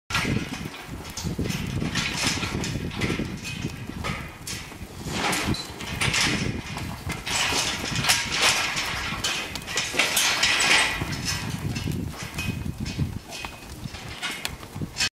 Carribean Senepols PTIC Cows For Sale
Carribean Senepols PTIC Cows for Sale - Full bull calf contract available, contact James Lilburne - Australian Topstock for more information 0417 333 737